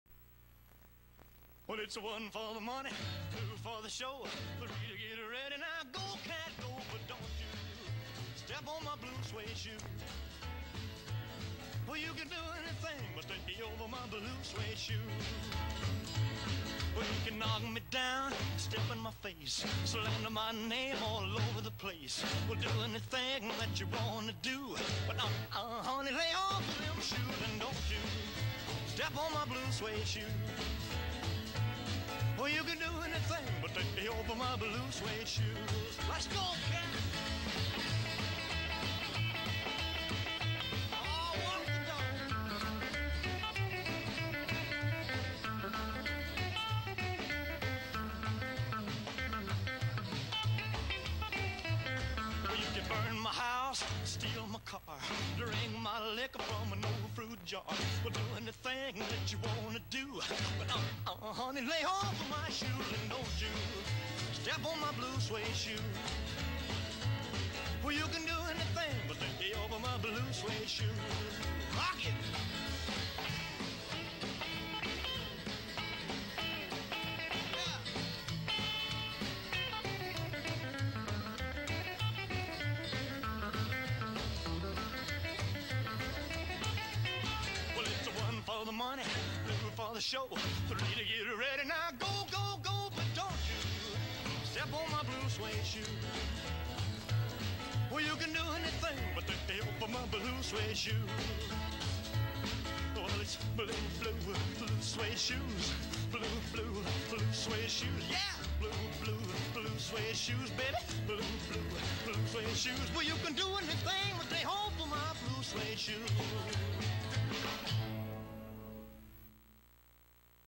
Dancing To Elvis
Mom, Dad and I in 1958!